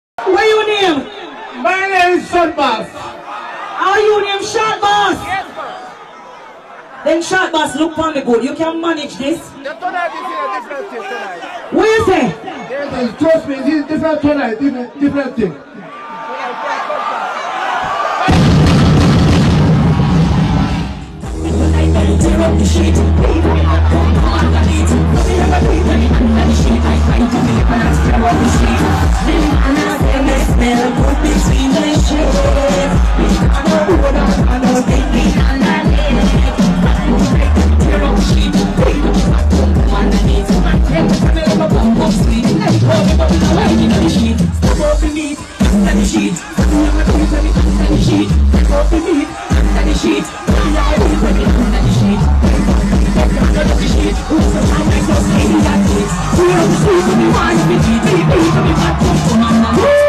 Spice goes wild with Gambia's short boss on stage
Spice goes wild with Gambia's shortest man on stage